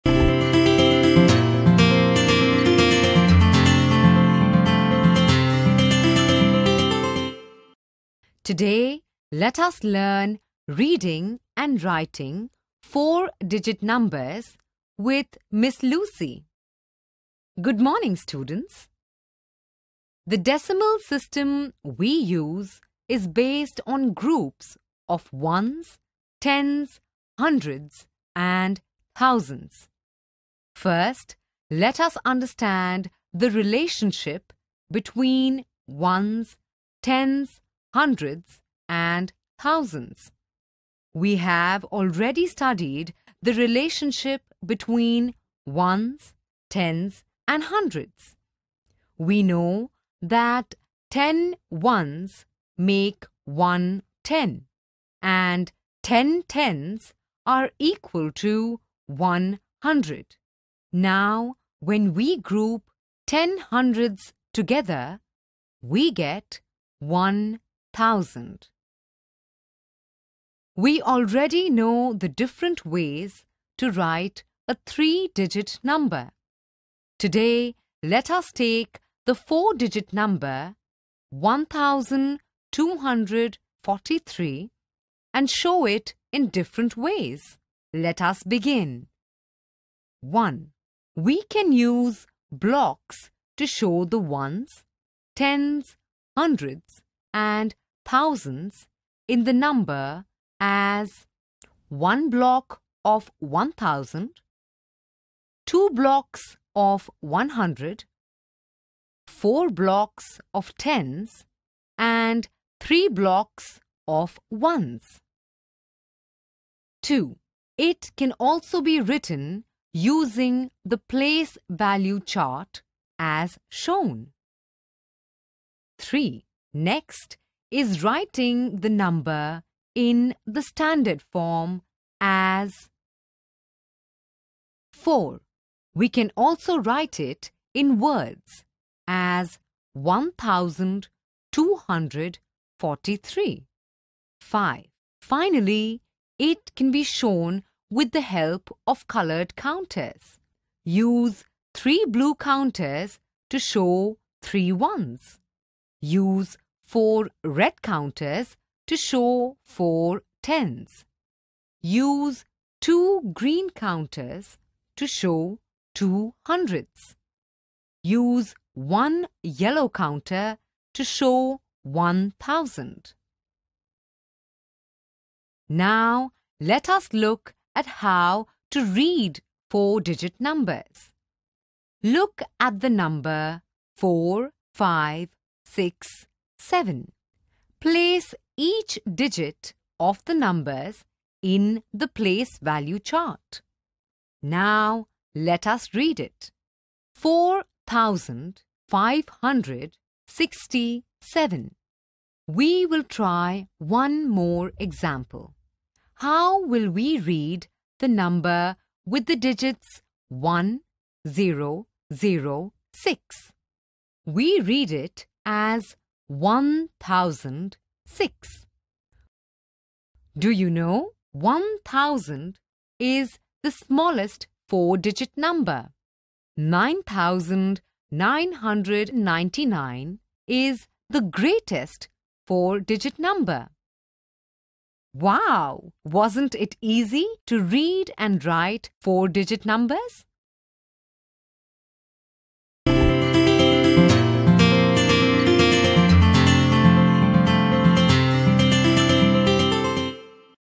Reading And Writing 4 Digit Numbers | Mathematics Grade 3 | Periwinkle
Today let us learn reading and writing 4 Digit Numbers with Miss Lucy.